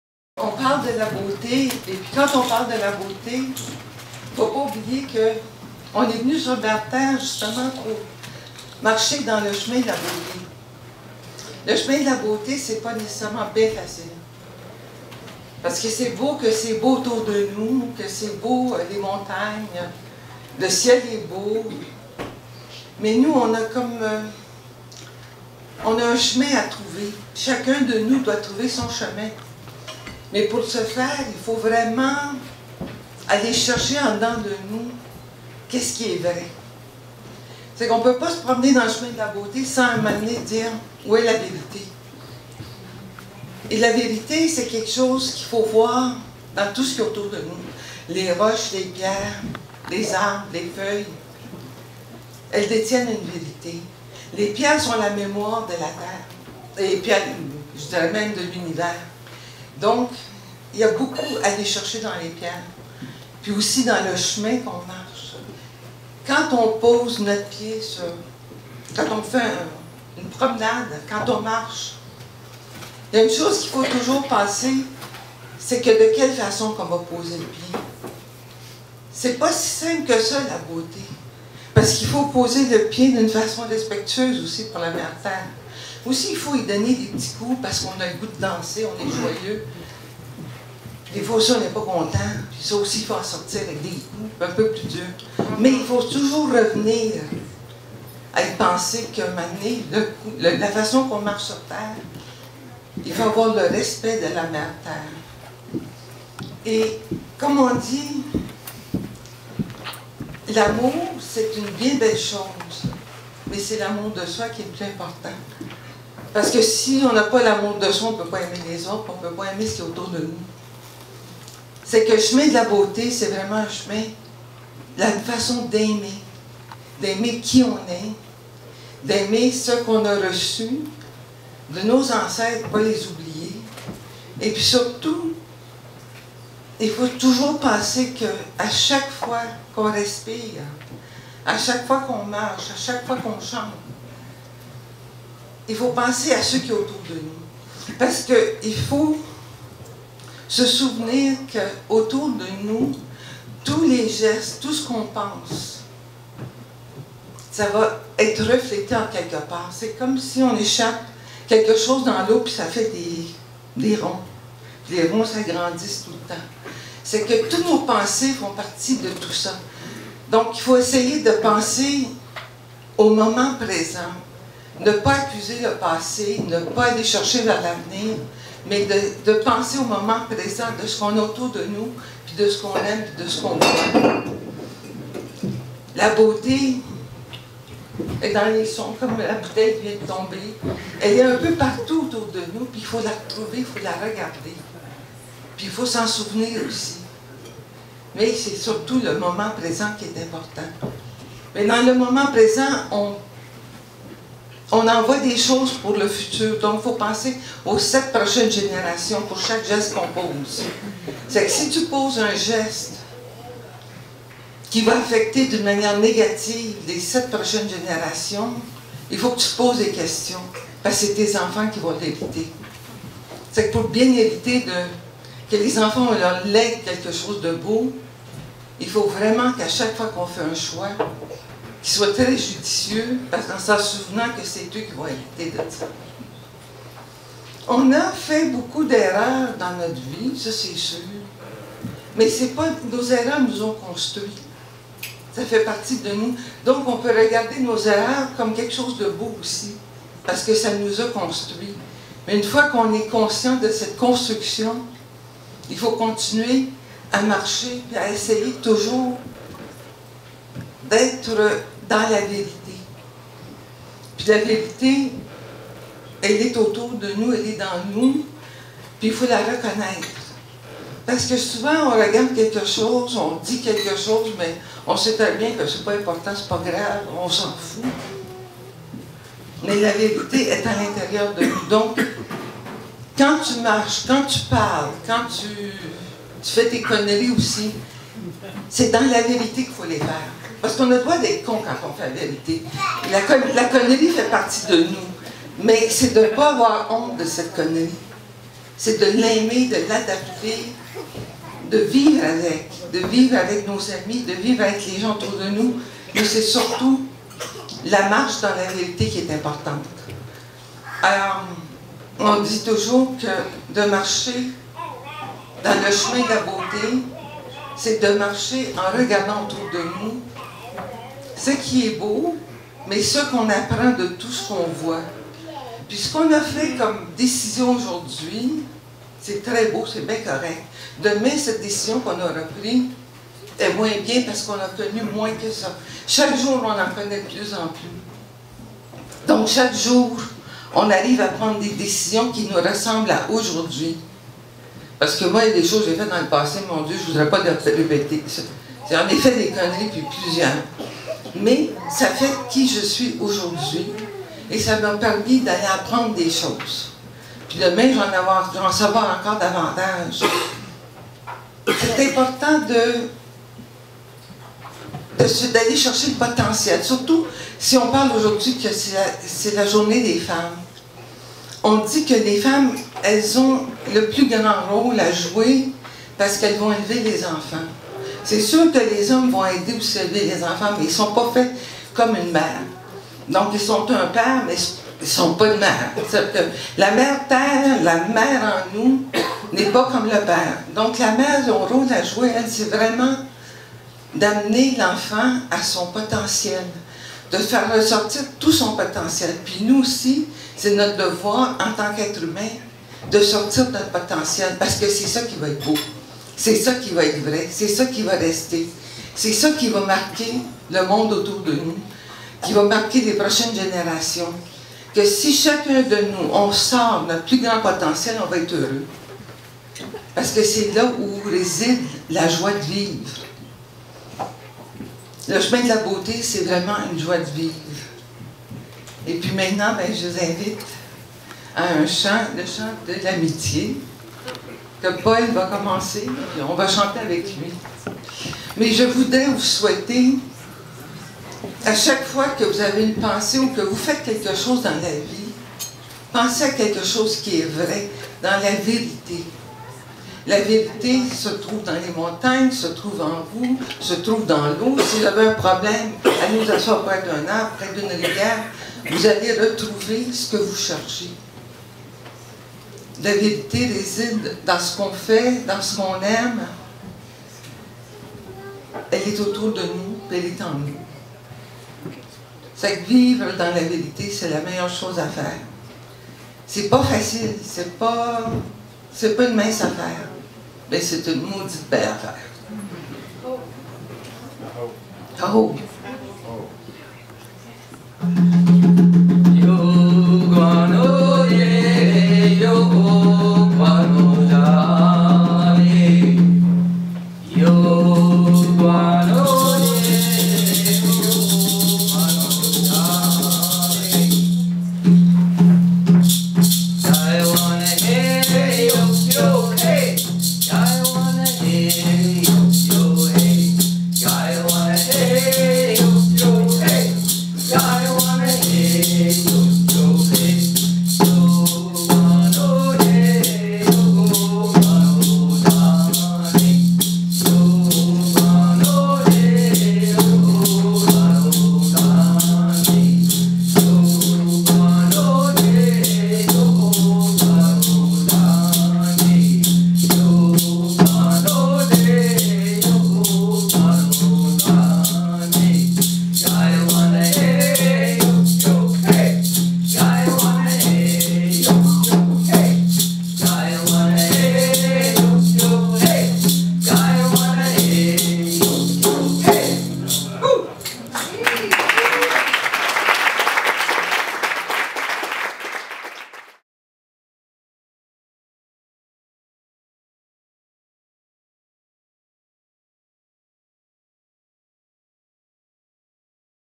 Le Chemin de la Beauté
Lors de «Miroir, Miroir», le 8 mars 2014, pour la 6e édition de la célébration de la Journée Internationale de la Femme à Sutton (salle Alec & Gérard Pelletier).

Cabaret organisé par le collectif «Les Tisserandes», qui cette fois ont décidé de s'amuser à tisser une courtepointe sur le thème de la Beauté. Imposée par les diktats, changeante selon les époques, et surtout, véritablement intérieure, la Beauté fait encore jaser en 2014.